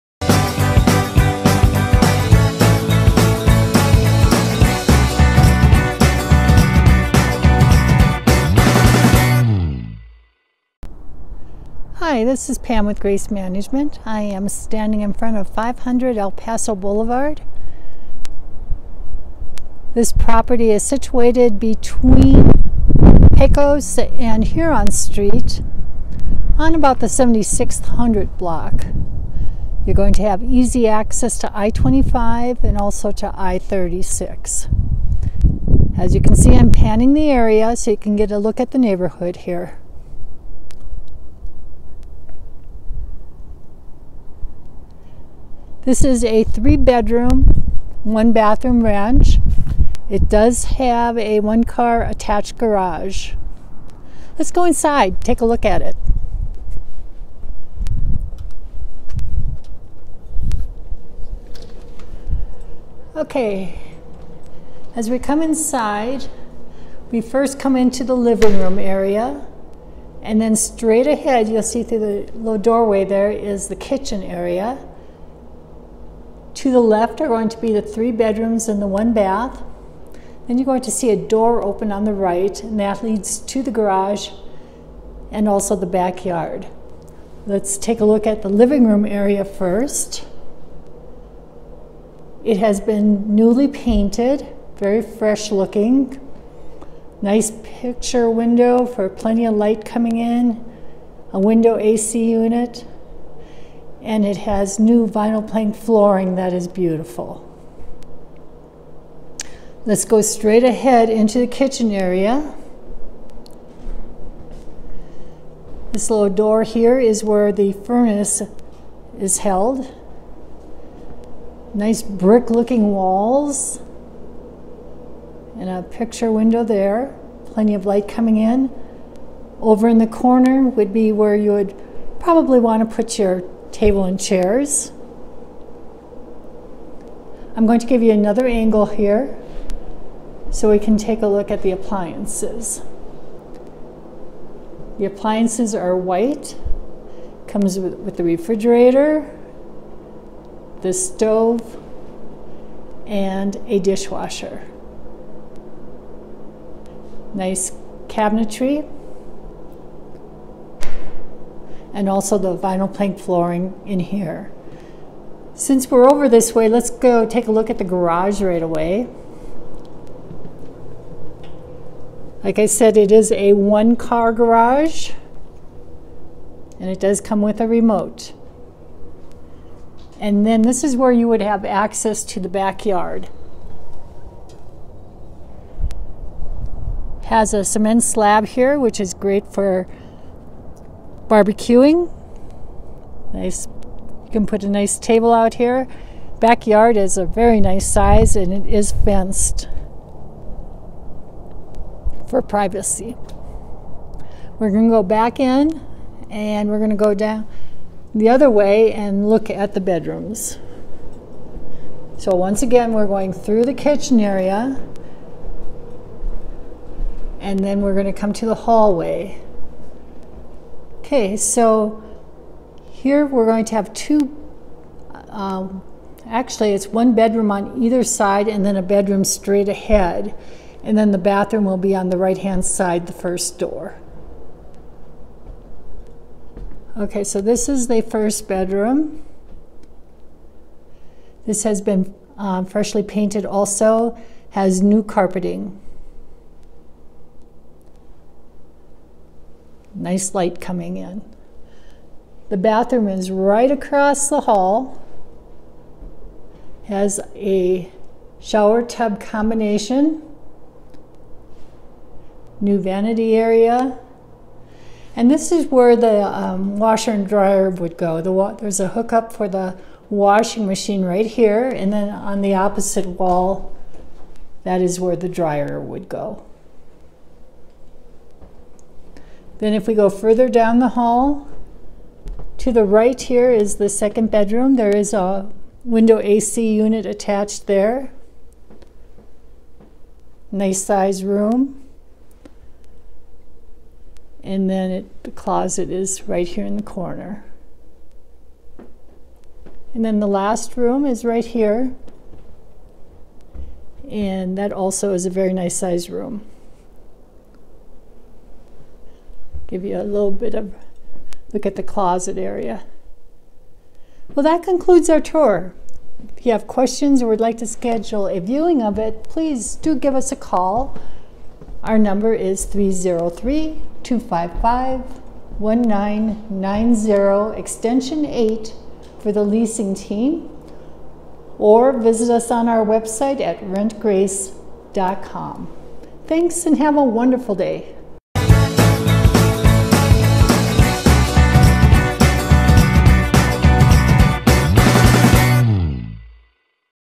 Denver Homes for Rent 3BR/1BA: 500 El Paso Boulevard by Grace Property Management & Real Estate
This great 3-bedroom home is a steal! With new luxury vinyl flooring, new carpet, fresh paint, clean and ready for move-in. The large living room at the entrance offers a picture window and plenty of sunshine. The eat-in kitchen features ample cabinet space, nice countertops, and necessary appliances. All three bedrooms offer nice-size closet space. There is an attached 1-car garage. The huge backyard has a patio area for grilling and some outdoor activities. Centrally located within minutes of I-25 and Hwy-36. Check this out and make it yours!

Looking for a property manager in Denver? We do property management!

2200 E 104th Ave UNIT 105, Thornton, CO 80233